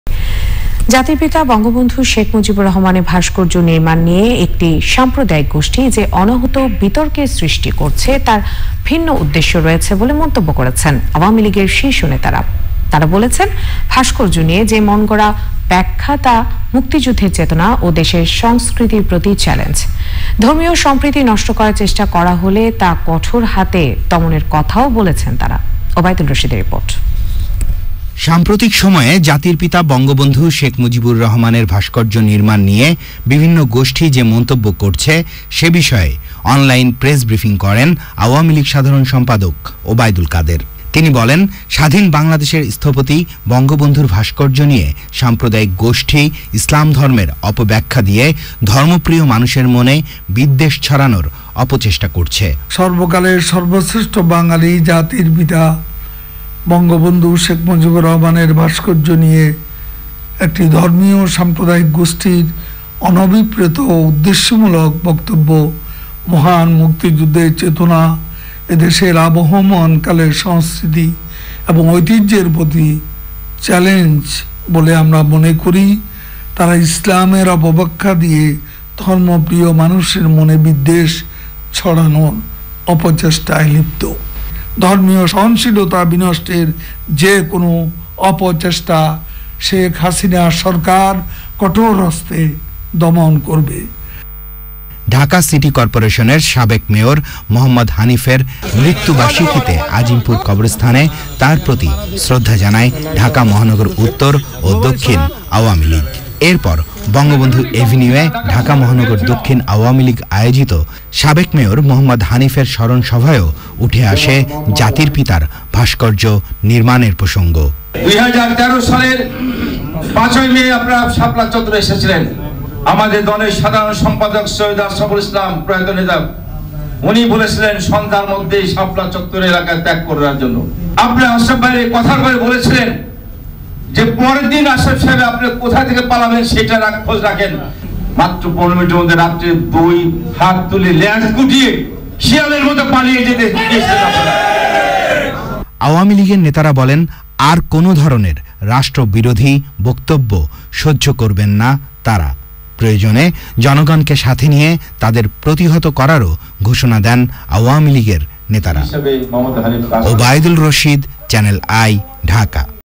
বঙ্গবন্ধুর ভাস্কর্য নির্মাণ নিয়ে বিরোধীতা সহ্য করা হবে না বলেছে আওয়ামী লীগ
জাতির পিতা বঙ্গবন্ধু শেখ মুজিবুর রহমানের ভাস্কর্য নির্মাণ নিয়ে একটি সাম্প্রদায়িক গোষ্ঠি যে অনাহুত বিতর্কের সৃষ্টি করছে তার ভিন্ন উদ্দেশ্য রয়েছে বলে মন্তব্য করেছেন  আওয়ামী লীগের শীর্ষ নেতারা। তারা বলেছেন, ভাস্কর্য নিয়ে যে মনগড়া ব্যখ্যা তা মুক্তিযুদ্ধের চেতনা ও দেশের সংস্কৃতির প্রতি চ্যালেঞ্জ। ধর্মীয় সম্প্রীতি নষ্ট করার চেষ্টা করা হলে তা কঠোর হাতে দমনের কথও বলেছেন তারা।

worldwide.